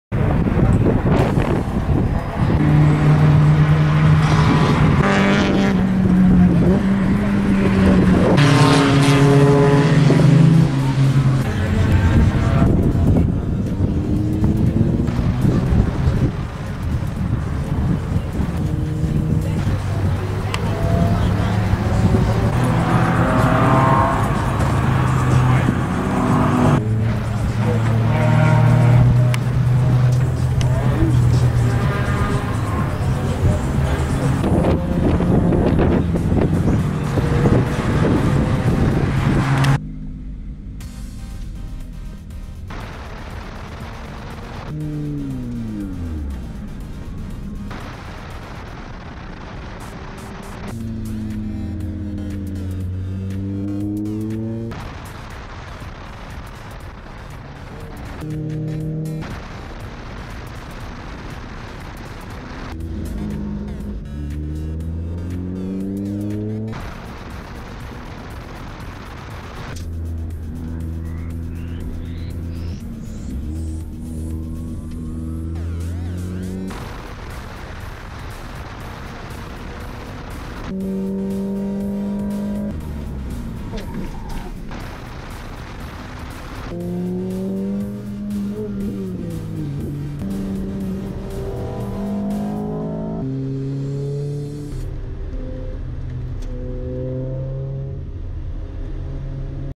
Racetrack Lurcy Lévis Dingos Gordos 2019
Chrono 1'18'77 en fin de journée sur bitume presque sec...  Pneus semi slicks Avon ZZR... Dingos Gordos au circuit de Lurcy Lévis le 06/10/2019 !
De nombreuses voitures présentes en voici quelques unes :
Renault Alpine, Lotus Exige, Bmw, R8 gordini, porsche